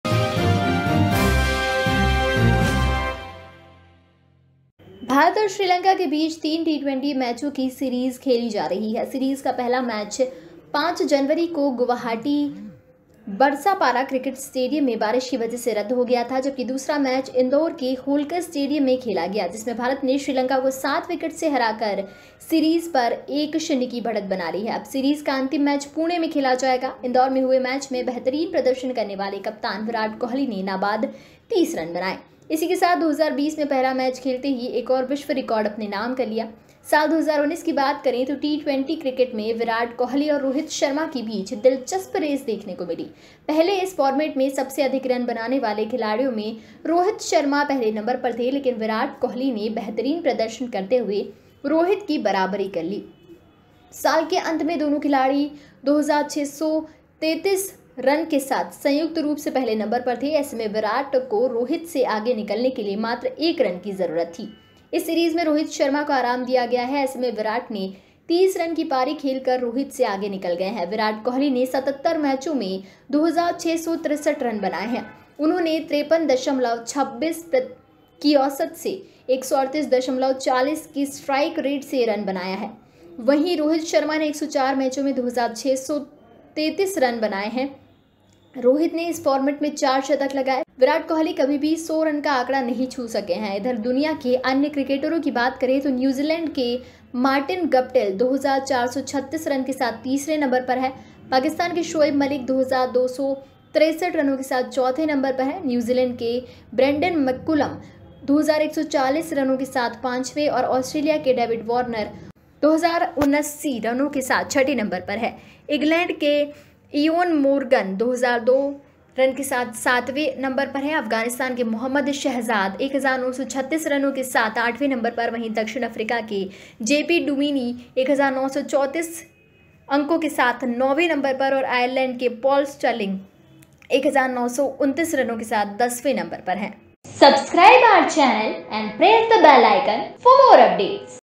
Ind Vs SL 2nd T20I : Virat Kohli ने Indore में कौन सा नया विश्व रिकॉर्ड बनाया | Ghamasan Live
कैसे तोड़ा Virat Kohli ने अपने ही दोस्त का रिकॉर्ड |